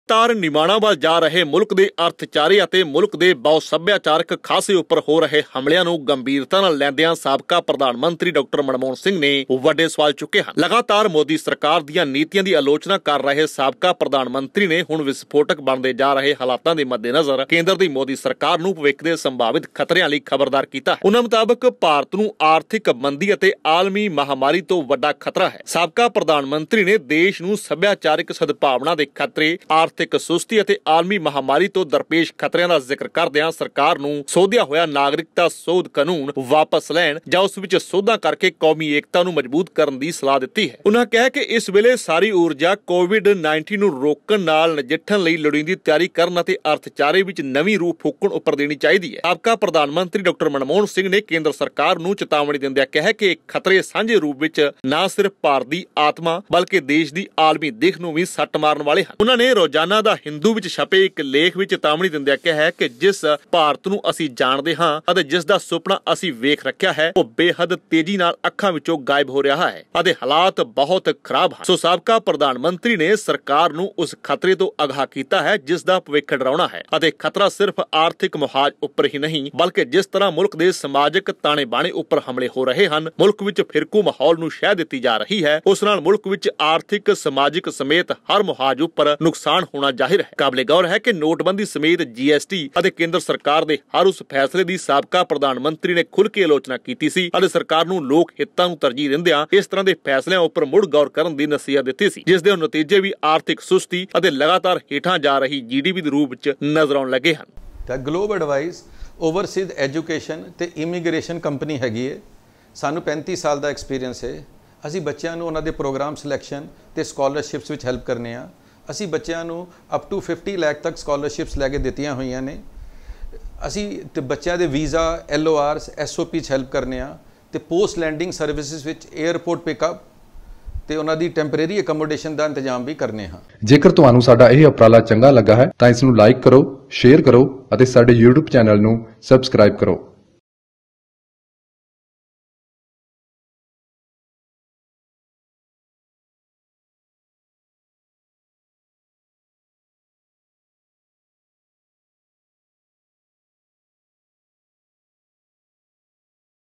Manmohan Singh Sees India "Slipping Away", Says Modi Must Reassure Nation || SIKH TELEGRAPH || SNE
Manmohan Singh Sees India "Slipping Away", Says PM Must Reassure Nation || SIKH TELEGRAPH || SNE